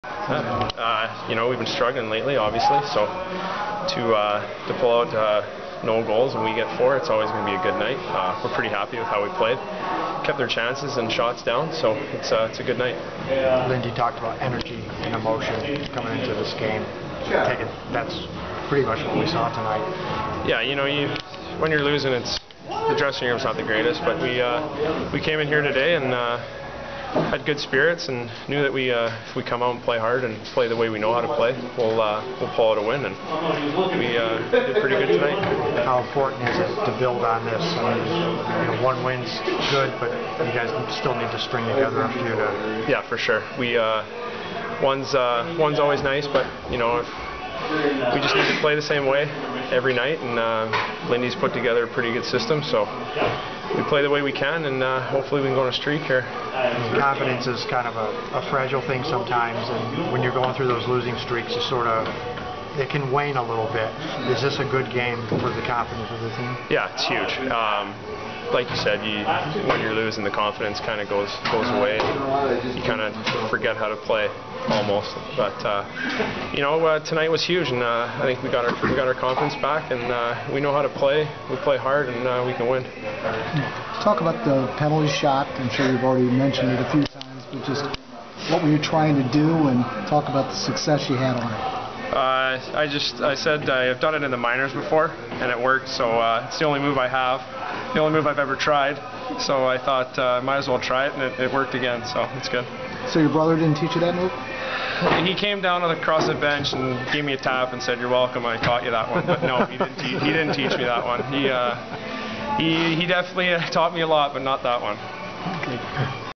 Jordie Benn On Making A Penalty Shot!
1-21-14:
The clincher to a 4-0 shutout this night over the Wild was a successful penalty shot by Jordie Benn. Brother Jamie says he taught him how to do that. Lindy Ruff says otherwise. Here's Jordie